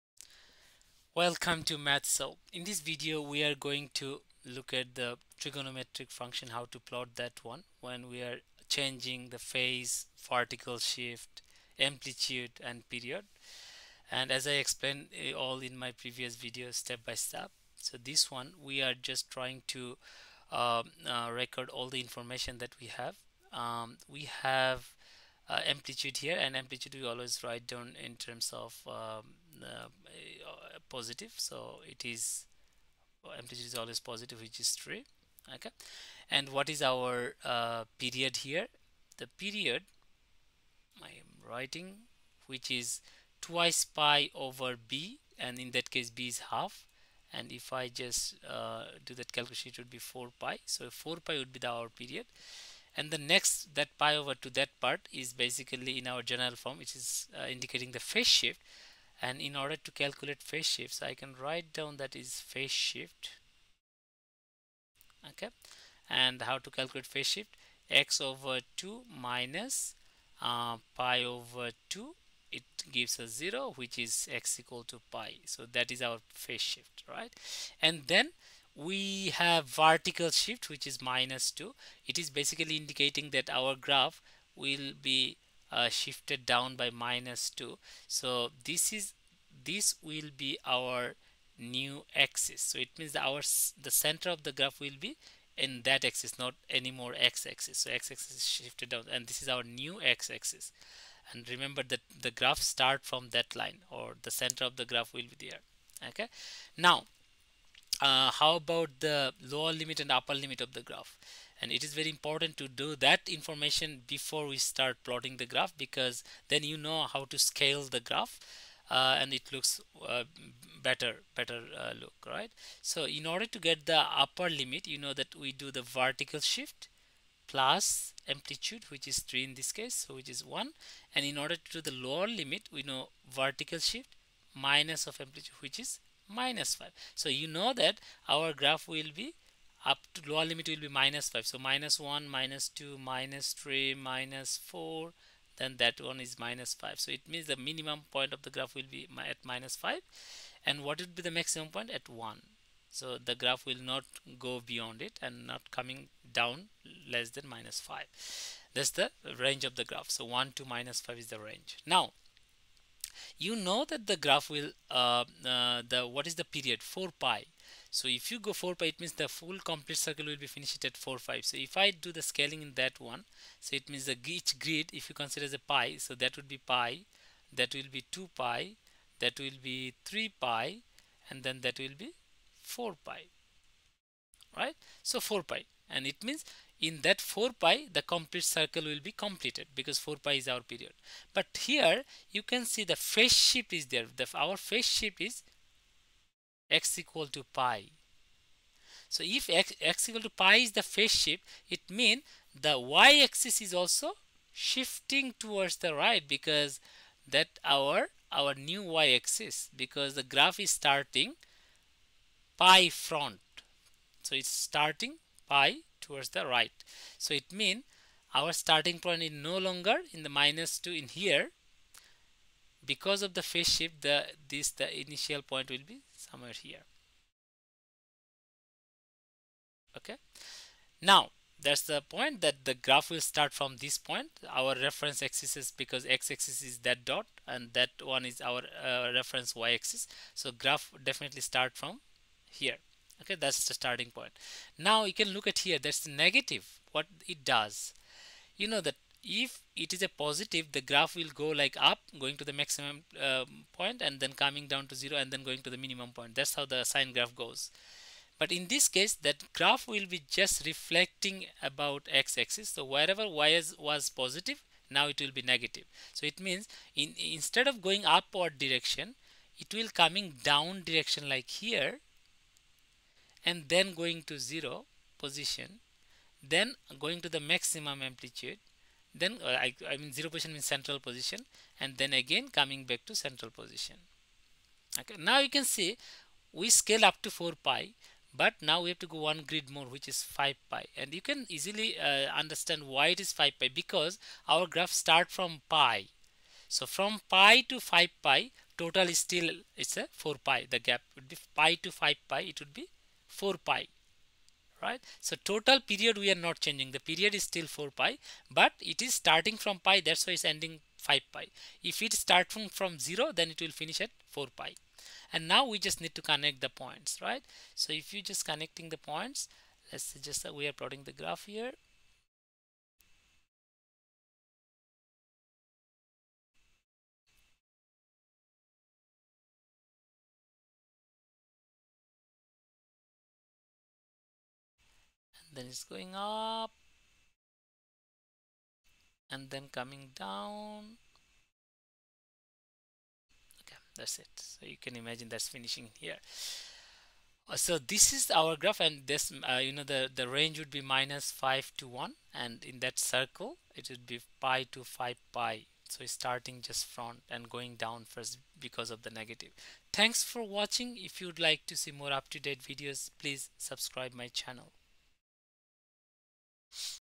Graphing Trigonometric function (amplitude, period, phase shift, vertical shift)
In this video we will see the modification of y = sin (x) by changing amplitude, period, phase shift, and vertical shift. Hope this will help.